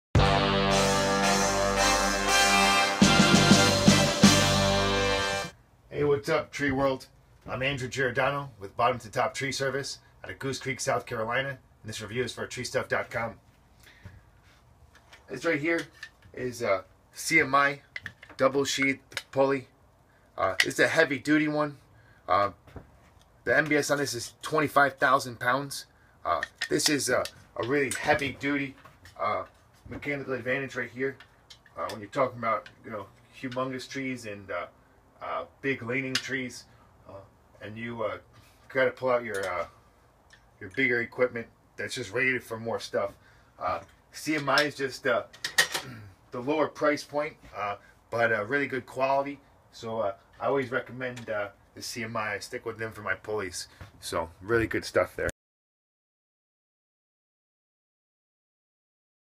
CMI RP124 Pulley: TreeStuff.com Customer Andrew Giordano's Review In The Field
South Carolina arborist Andrew Giordano gives us his Review In The Field for the CMI RP124 pulley.